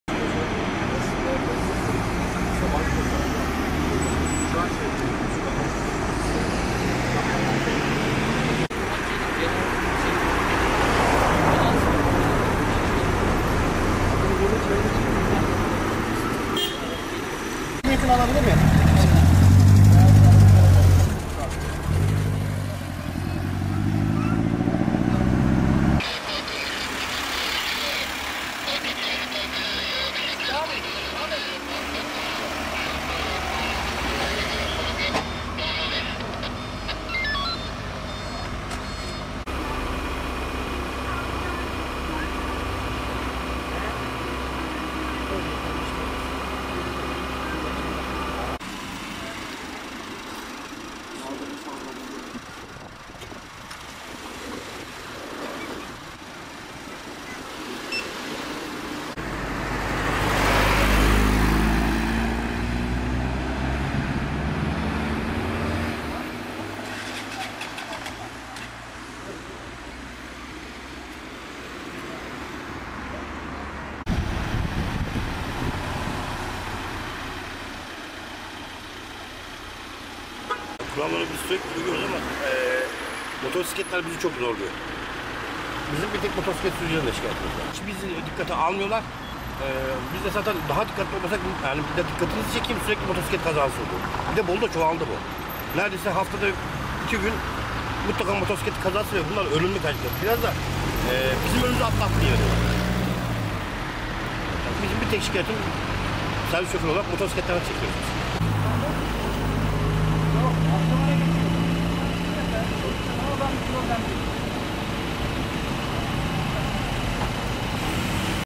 831
Bolu'da trafik denetimleri yoğunlaştı. Karaçayır Mahallesi'nde şok denetimler gerçekleştirildi. Trafik kurallarına uymayan sürücülere ceza uygulandı, özellikle motosiklet sürücüleri hedef alındı.